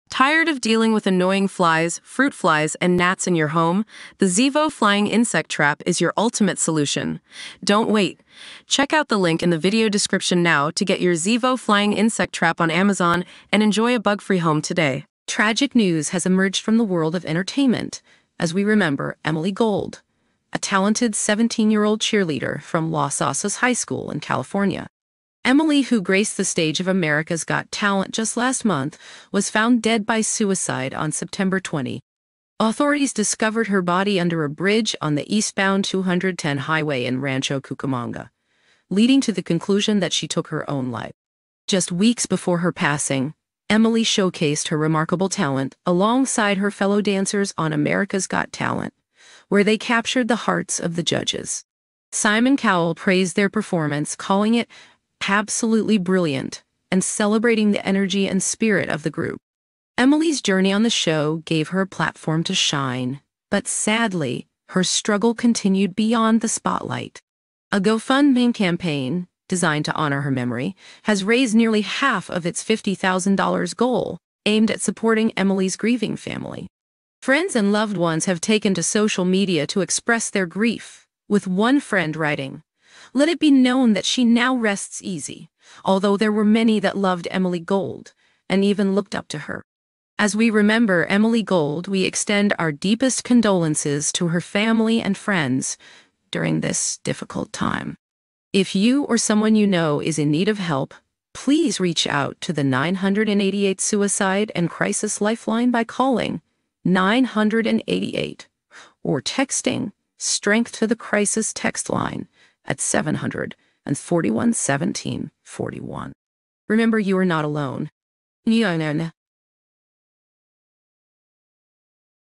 Tragic Loss: Remembering Emily Gold from America’s Got Talent
In this heartfelt video, we pay tribute to Emily Gold, a talented 17-year-old cheerleader from Los Osos High School in California, who recently appeared on America’s Got Talent. Tragically, Emily was found dead by suicide on September 20, just weeks after impressing judges with her dance team. We also emphasize the importance of mental health and encourage anyone who is struggling to reach out for help. Our thoughts and condolences are with Emily's family and friends during this difficult time.